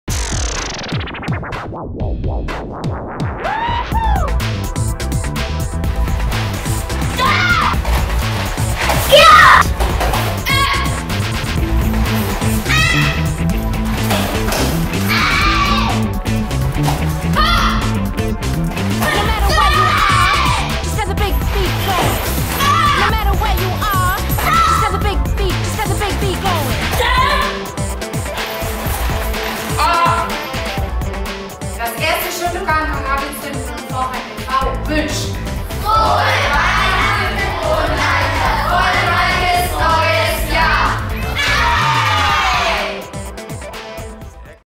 Weihnachtsgrüße 1. Shotokan - Karate - Zentrum Forchheim e.V.
Das 1. Shotokan - Karate - Zentrum Forchheim e.V. wünscht mit seinem Nachwuchskader unter Trainerin Silvia Schnabel allen Sportlerinnen und Sportlern, Eltern, Betreuern und Freunden friedliche Weihnachten, einen gelungenen Jahreswechsel und ein gesundes Neues Jahr!